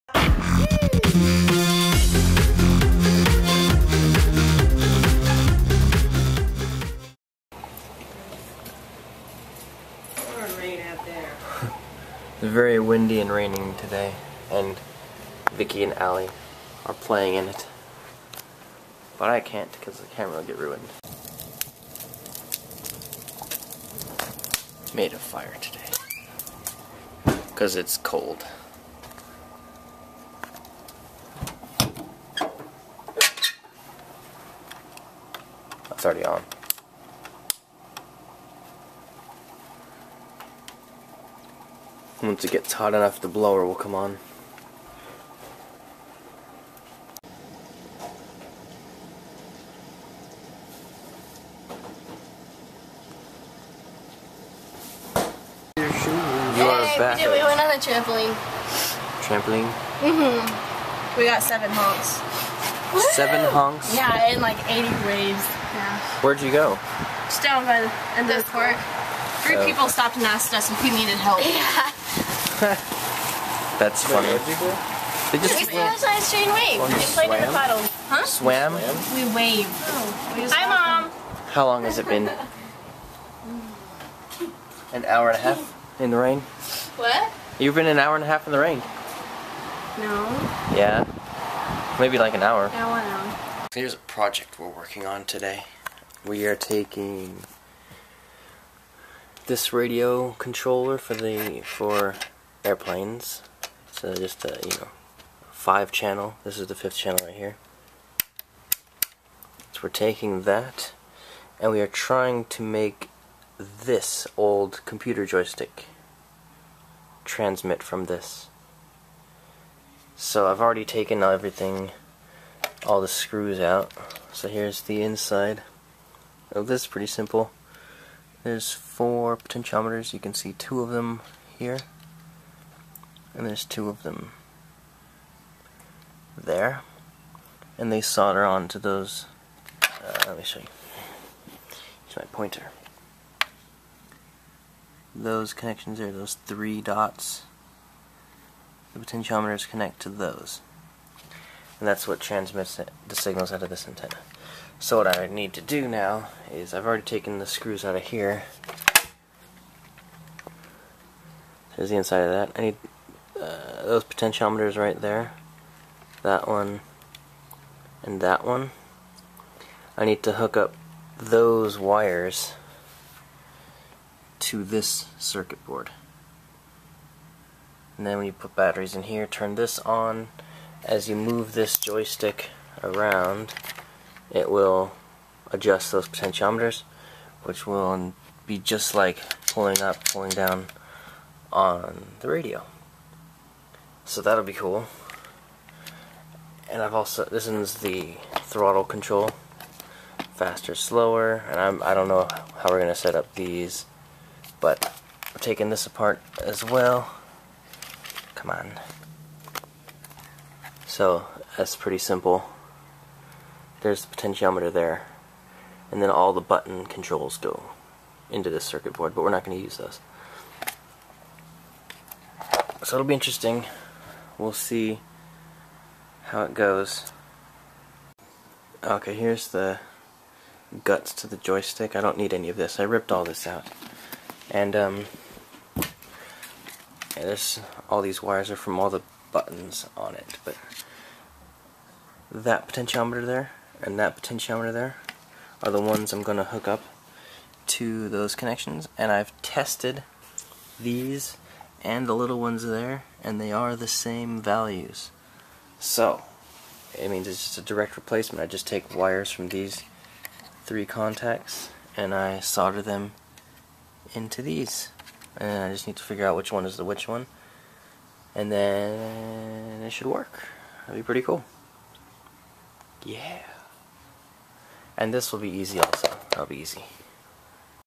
[152] Playing in the Rain, Modifying R/C Plane Radio - 2010-10-24
It rained today, a lot. My sister and cousin played in it, we start a fire, and start working on converting an old pc joystick into a radio for r/c planes.